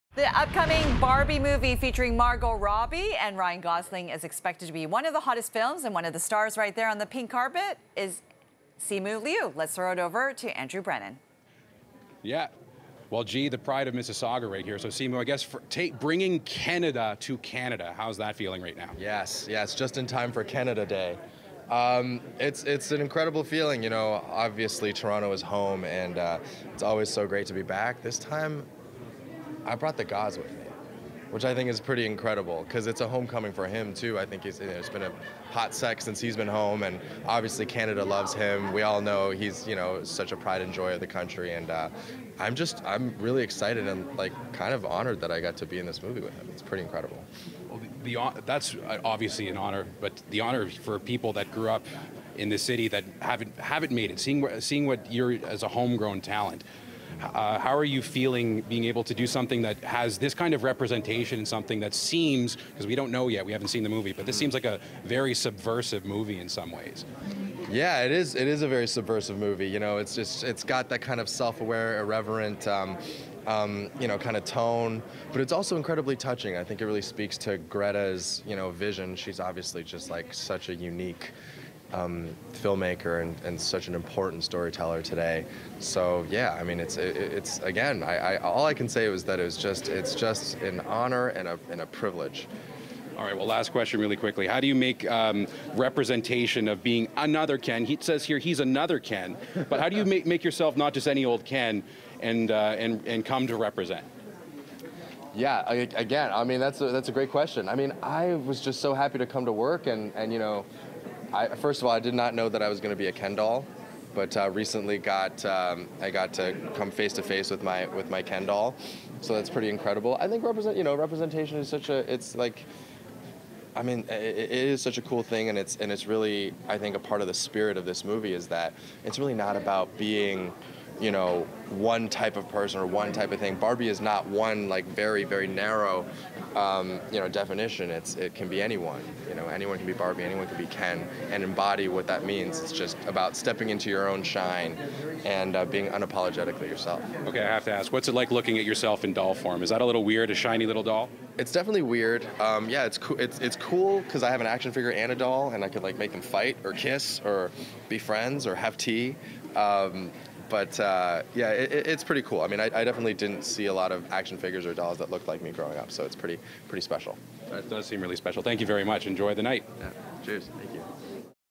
Simu Liu on his role in 'Barbie' movie
Simu Liu talks about the new 'Barbie' movie at a red carpet event in downtown Toronto.